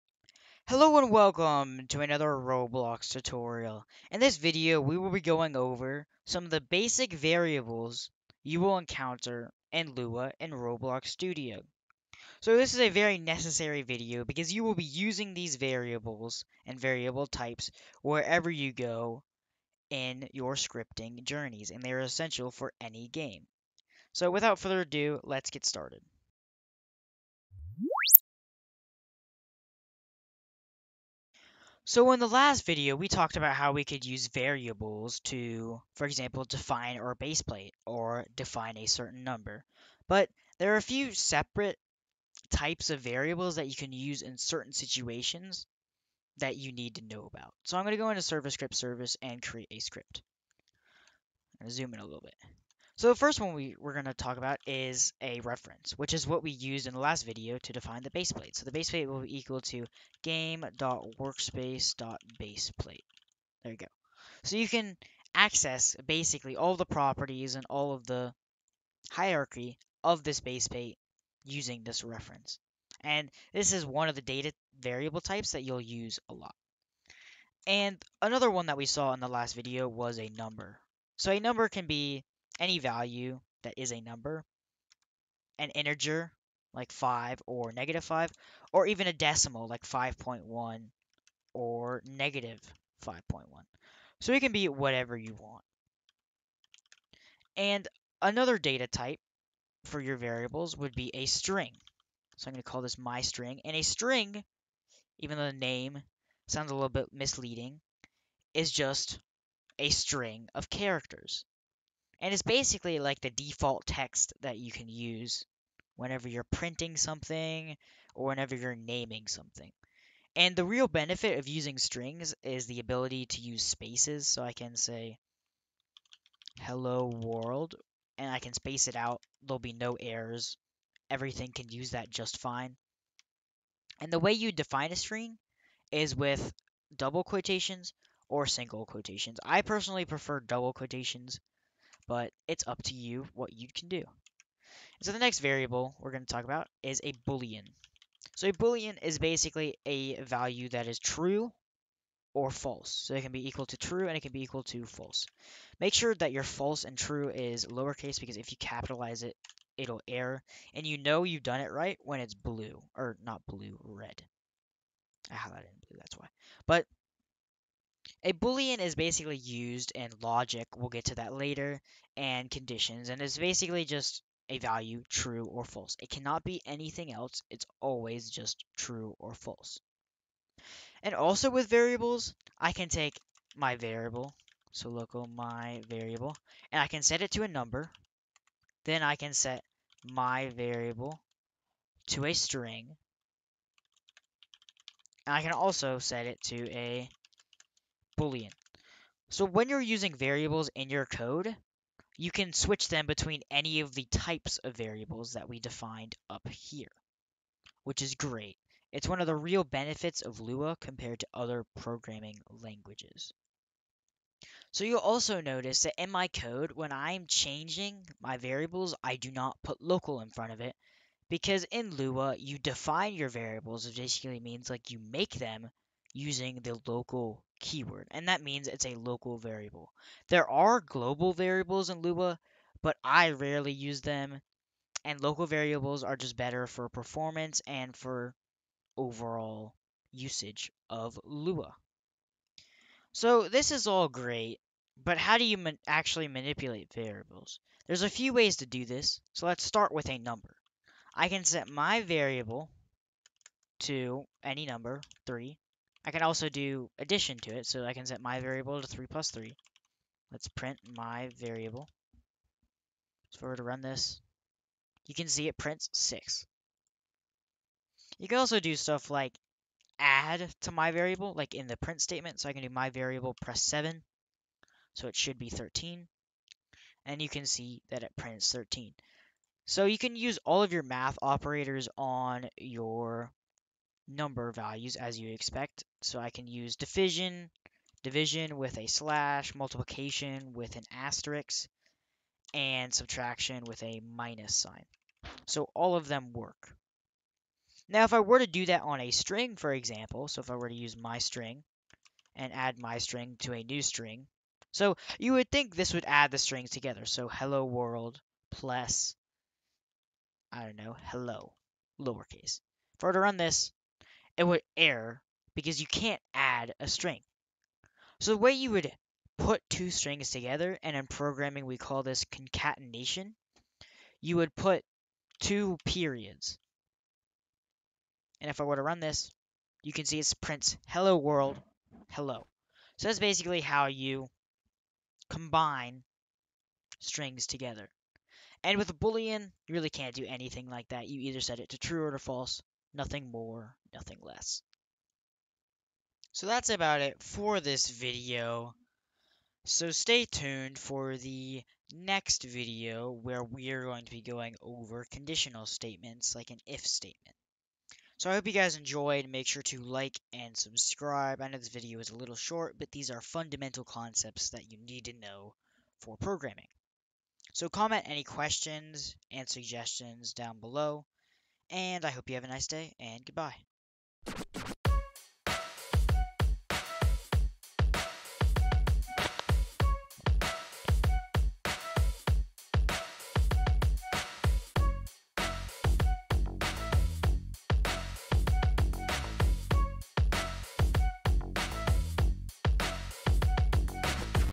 How to SCRIPT on Roblox #2 - Variables
In this video, I talk about the types of variables, such as numbers, strings, and booleans. This video is kind of dry, but it is essential knowledge for the upcoming videos. Hope you Enjoy!

Check out some more Roblox Content!

MAn5FqdtZIG-3Gsp4z

VaMQKxAdv0BA-LoUryn